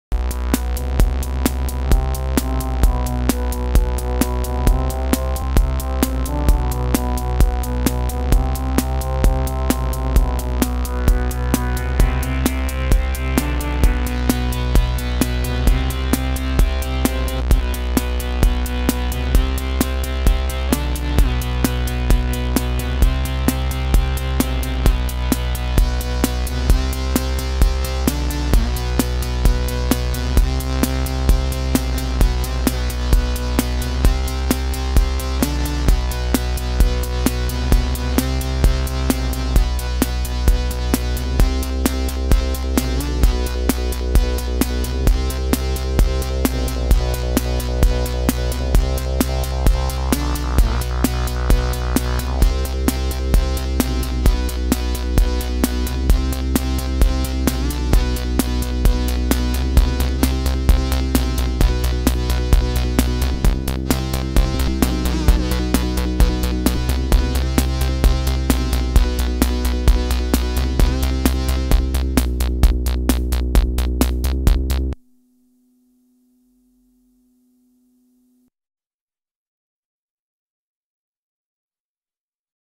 Korg monotribe and Yamaha reface cs - soundcheck
Audio recorded with a zoom u-24 (onscreen).

Video recorded with an iPhone 6.